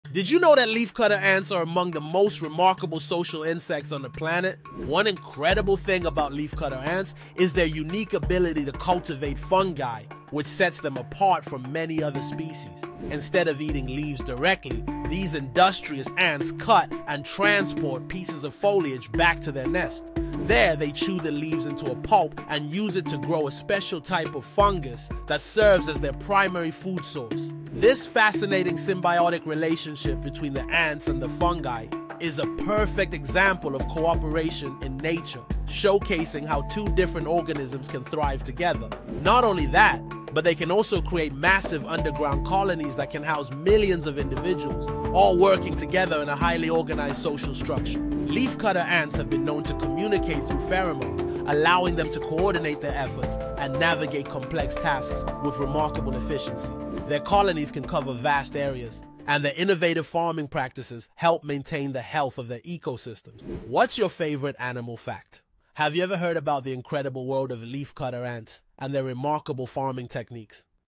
Leafcutter Ants: Nature's Farmers
Did you know leafcutter ants are master farmers? Discover their incredible world as they cultivate fungi!